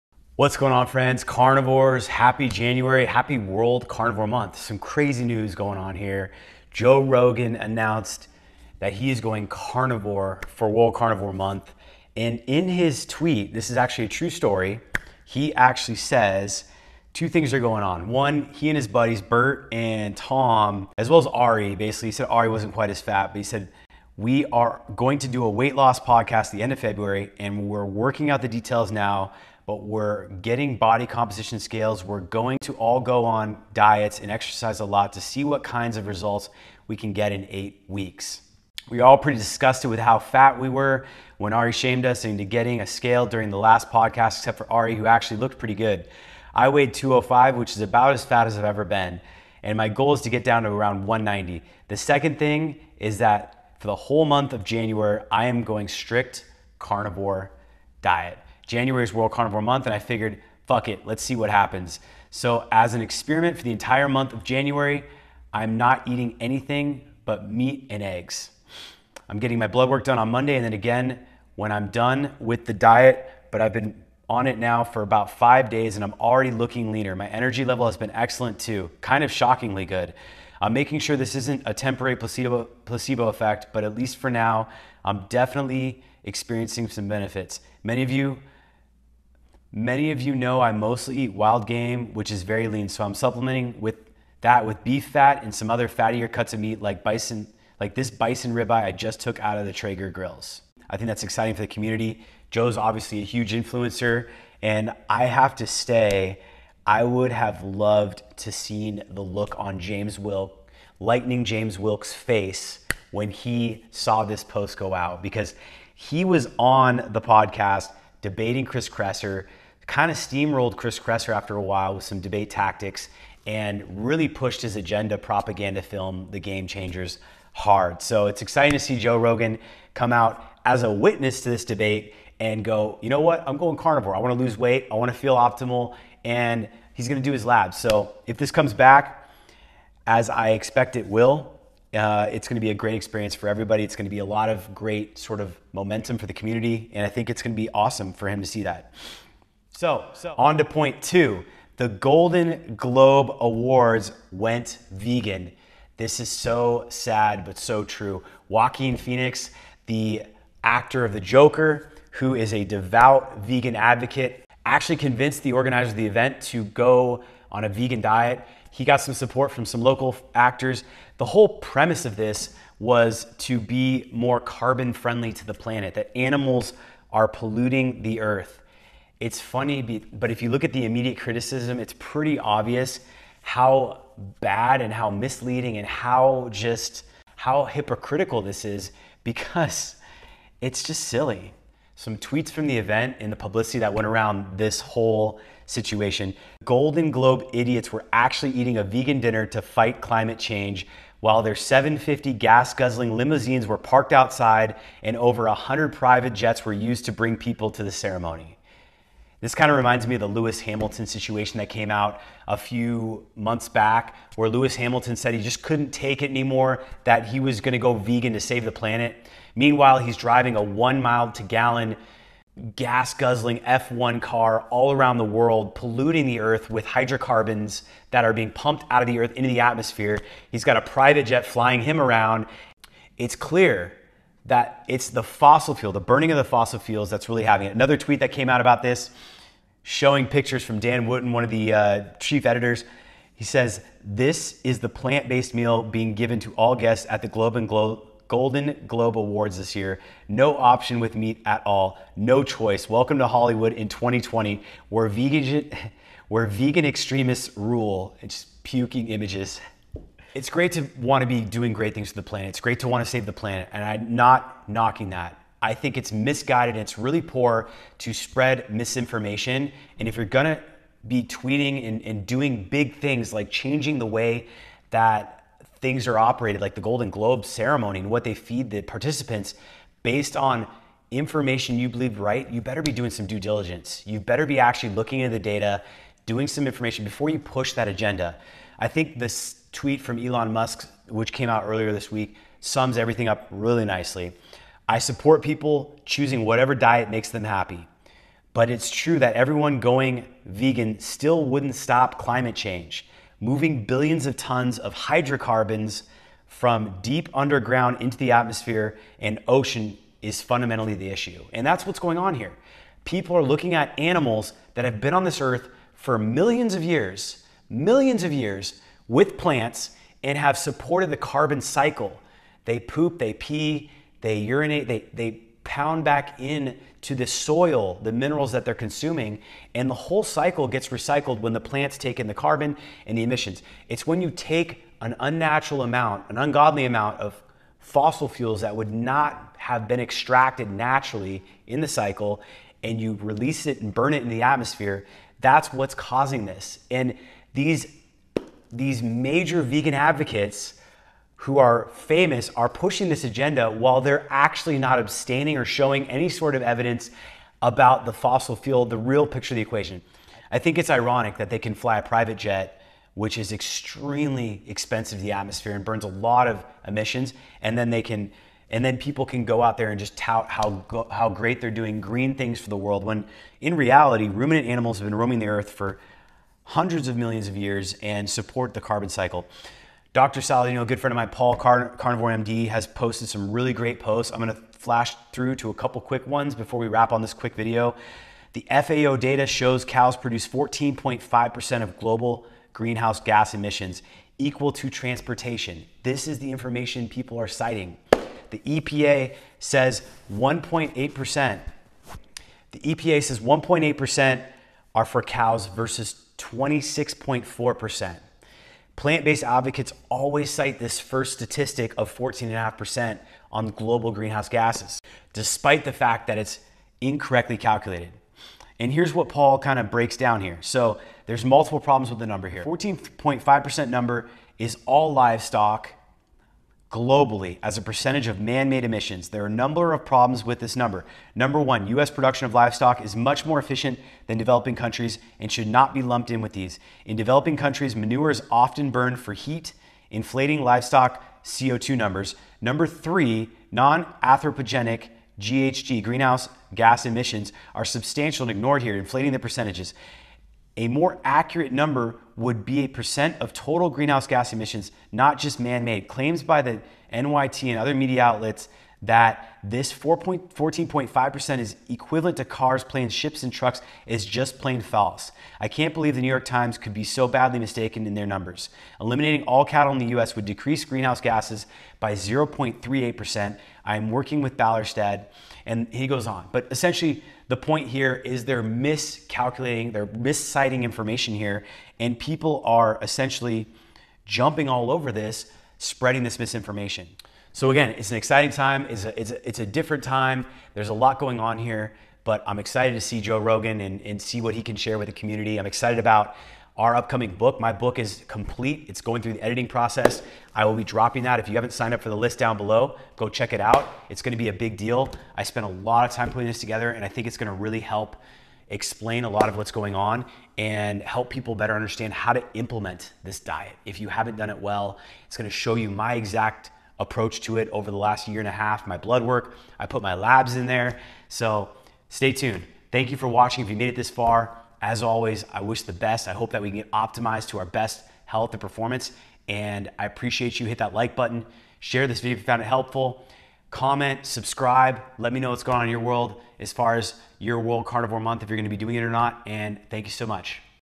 Joe Rogan is Going Carnivore | Golden Globes Go Vegan | Elon Tweets
There's been a flurry of news lately and it's really taken the public and media by storm.

First off welcome to World Carnivore Month! Joe Rogan officially announced that he's going strict Carnivore for the entire month of January. He's going to pull his blood work before and then after, but he's already stated he's feeling better.

The Golden Globes served a strict Vegan menu, thanks to the Hollywood vegan extremists who are doing their best to save the planet by arriving in their private jets and 750 limos they continue to demonstrate their intelligence and spread a false narrative that animals and animal farming is decimating the planet.

Even Elon had something to say in this one, and as you'll see the fundamental issue is fairly straight forward.

Check out the below to get more involved in my content, we are releasing a book very soon that will walk you through the entire process A to Z and make sure you have everything you need to know with a detailed program.

Wanna Catch All The Content? Sign Up For The Newsletter